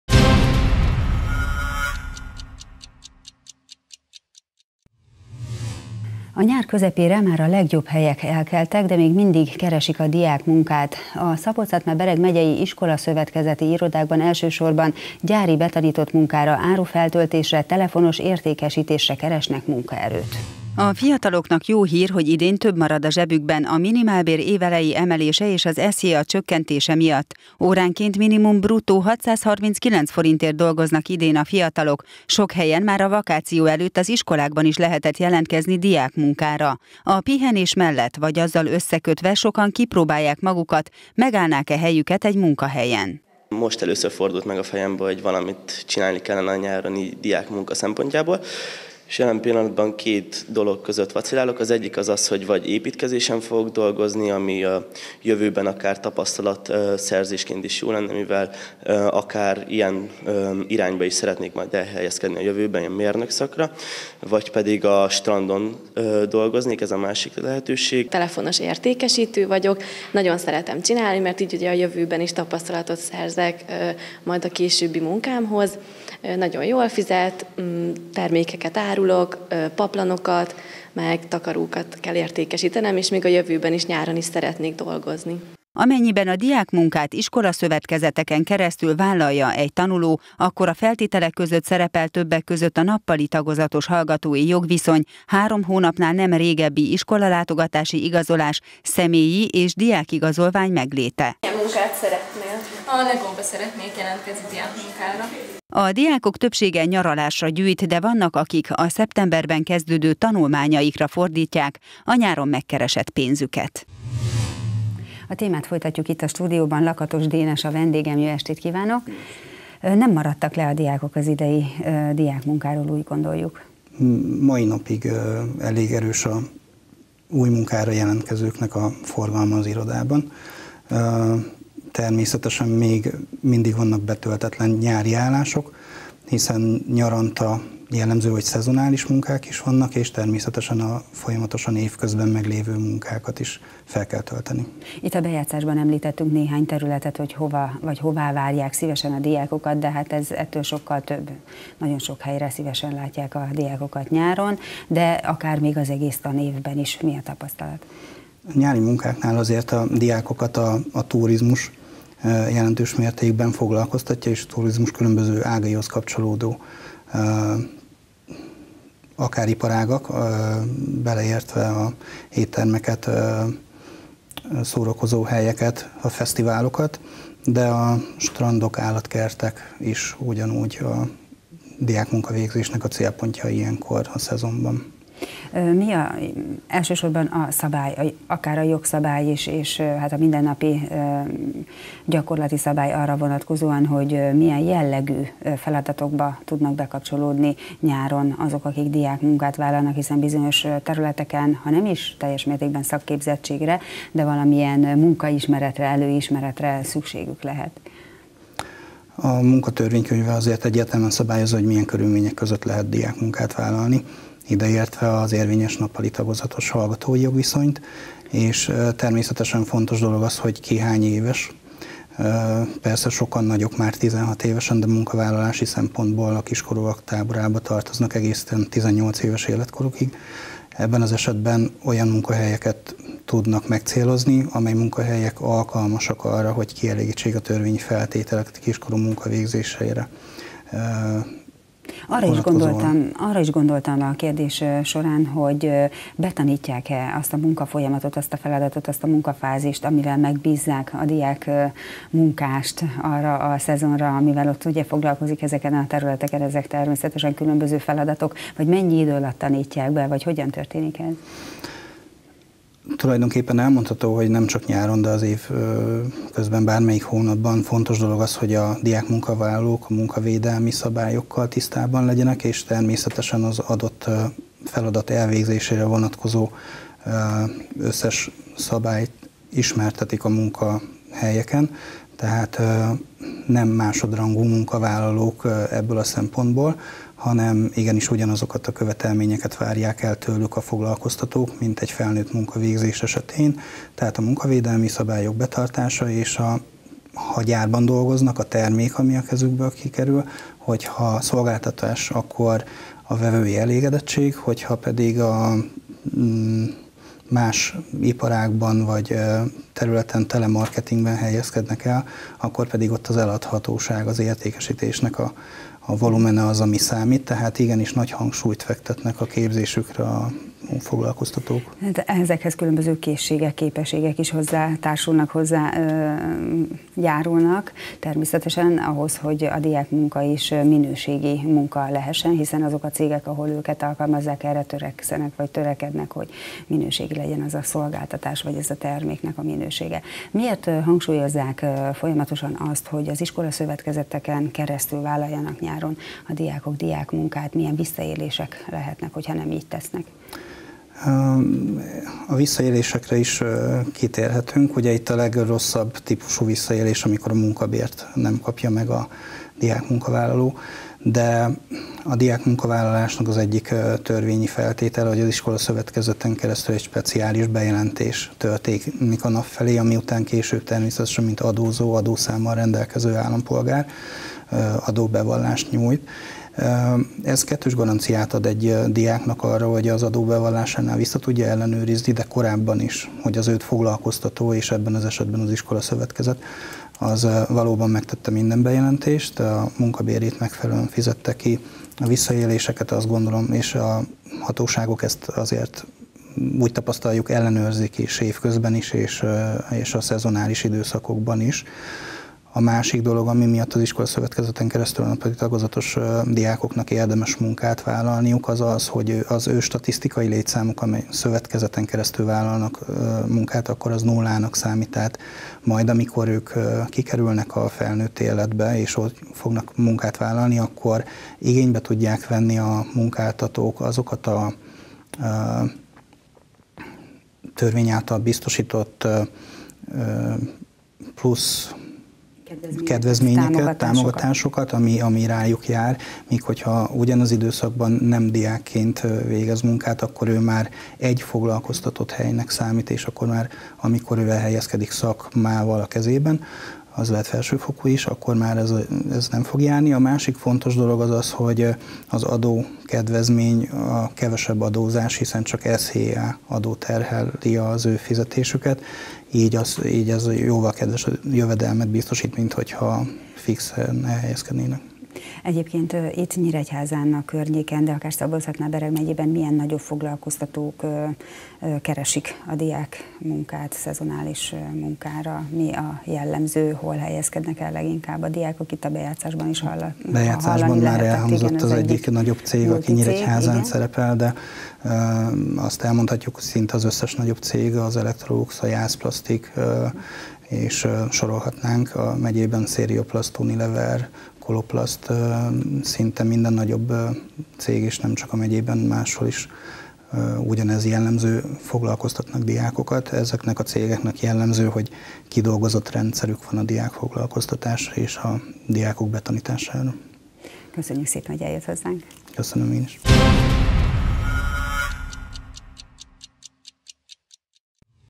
Dübörög a diákmunka
A nyár közepére már a legjobb helyek elkeltek, de még mindig keresnik a diákmunkát. A Szabolcs-Szatmár-Bereg megyei iskolaszövetkezeti irodákban elsősorban gyári betanított munkára, árufeltöltésre, telefonos értékesítésre keresnek munkaerőt.